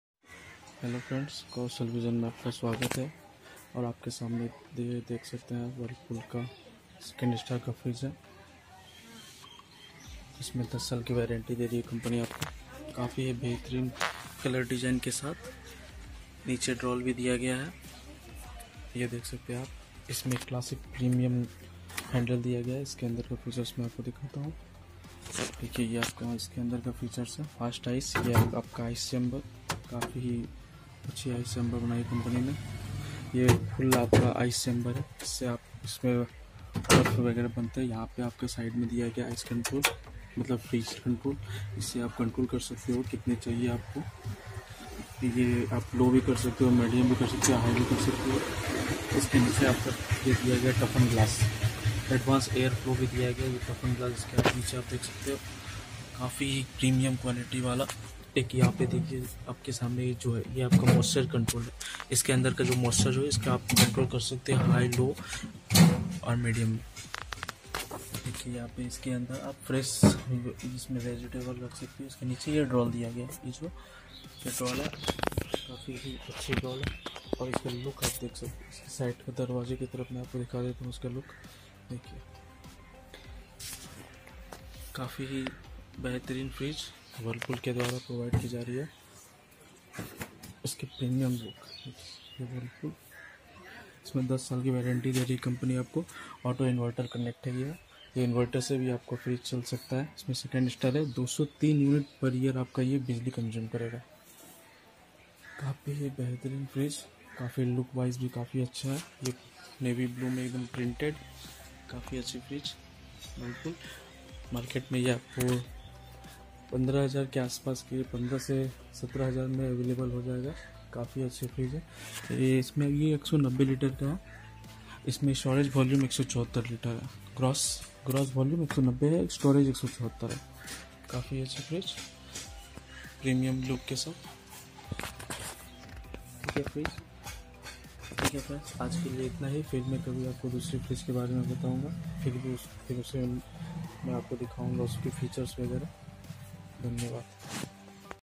Whirlpool refrigerator with best features..10 year warranty on product...
Whirlpool refrigerator..
10 year warranty on product ..
1 year full body  Warrenty and 9 year only on compressor